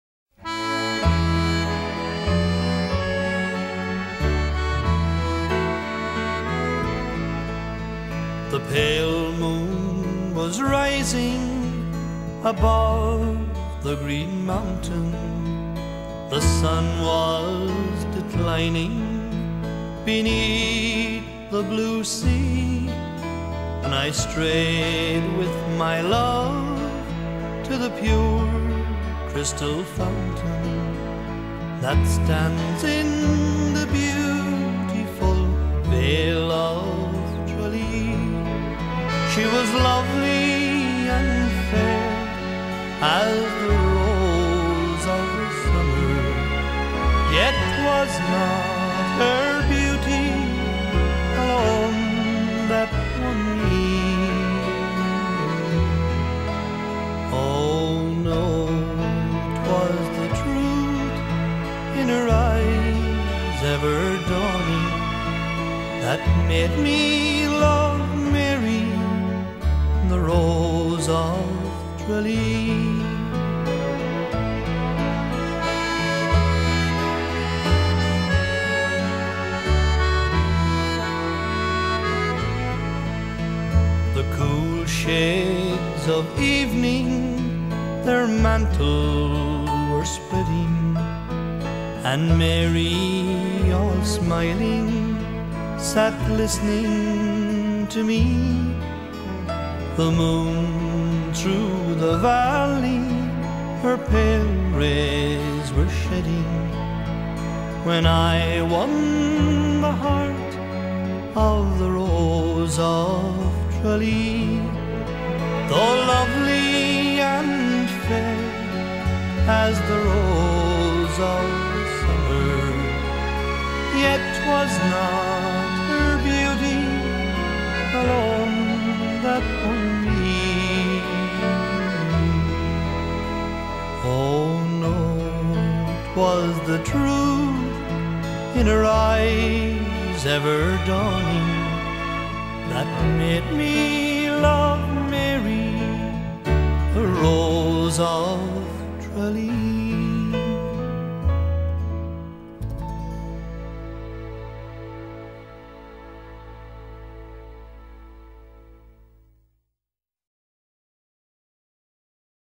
Old Photographs of Tralee c1900.
Glass plate images from the digital collection of The National Library of Ireland.